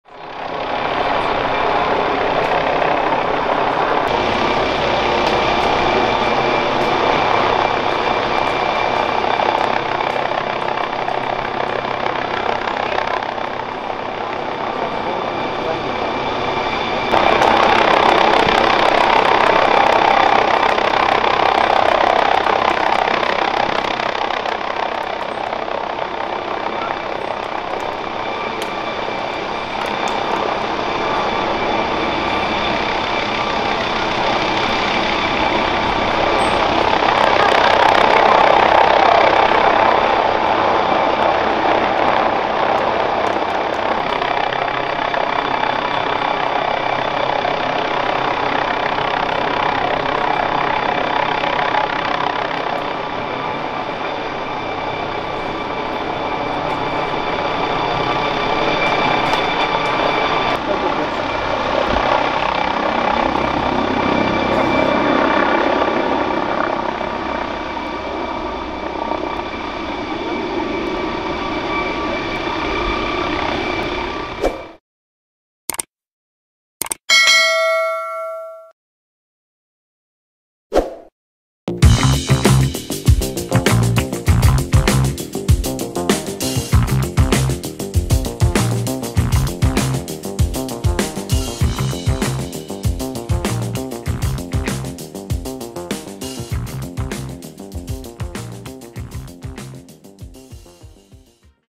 Belgian Air Force Days 2023 - MD Helicopters MD900 Explorer G-11 Belgian Federal Police
Summary of this video:
00:00  Belgian Federal Police hovering above crowd
01:14 Subscribe Dotwood Media - Thank you!

Video gear:
Sony FDR-AX33

© Dotwood Media 🔜🎥